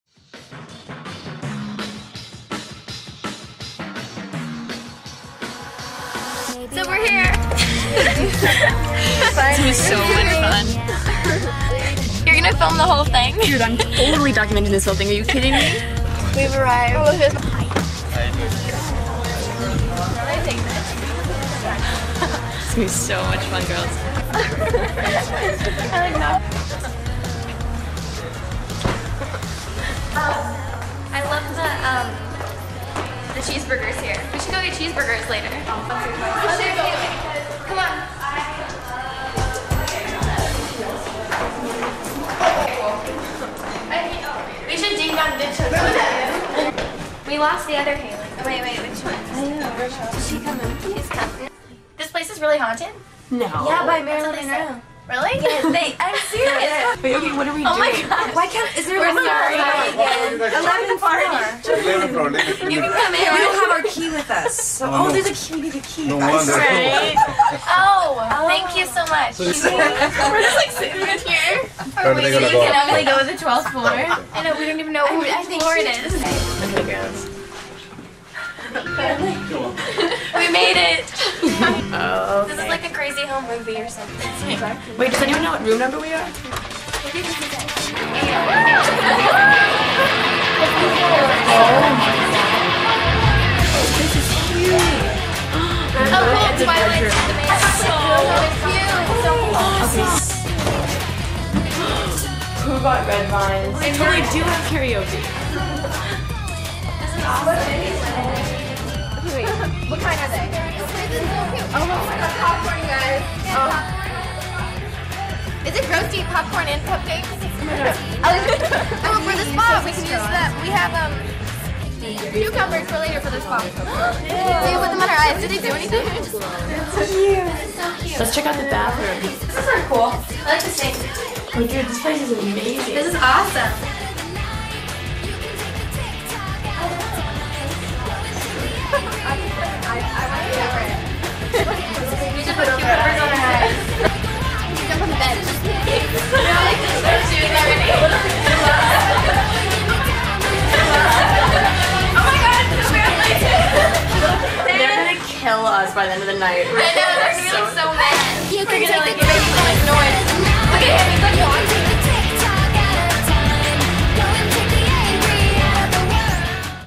Sleepover at the Roosevelt part 1
My friends and I thought it would be fun to stay at the Roosevelt Hotel in Hollywood for an all girls sleepover.  We all had so much fun! This is a little video from our night there.